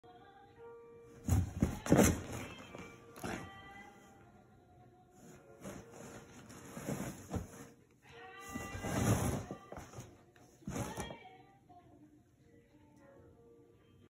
Playful Maine Coon Cat | Rolls out of Cardboard Box and Attacks Toy
Bear is a classic silver polydactyl maine coon cat. Bear is six years old and still as playful as a kitten lol.